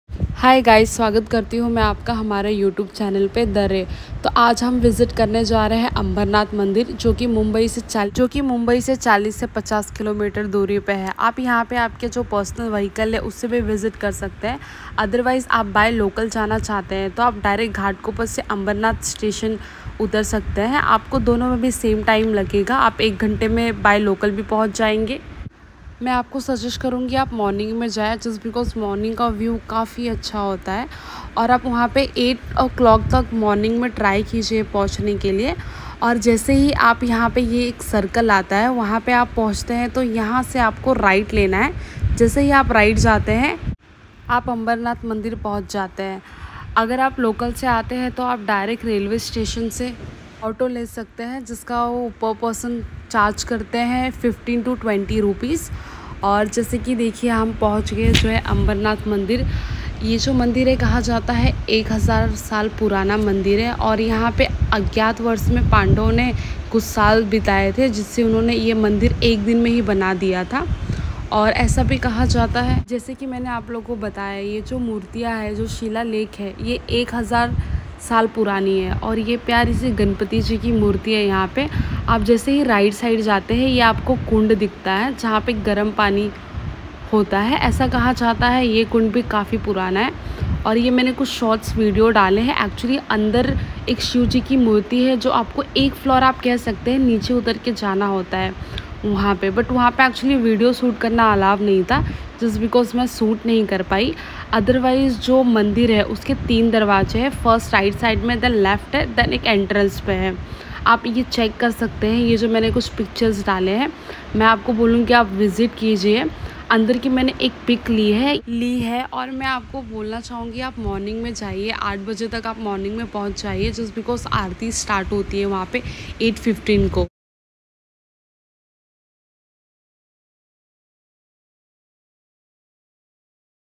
Amarnath Shiv Mandir 🛕
Hey guys,
Welcome to my YouTube channel The Ray today I visit Ambernath mandir it’s 40 -50 km from Mumbai so pls visit this place to see beautiful art work and you want to go for Arti so you can visit morning 8:15 am hope you like this video.

Address

new bhendi pada, Bhaskar Nagar (East, Deepak Nagar, Ambernath, Maharashtra 421505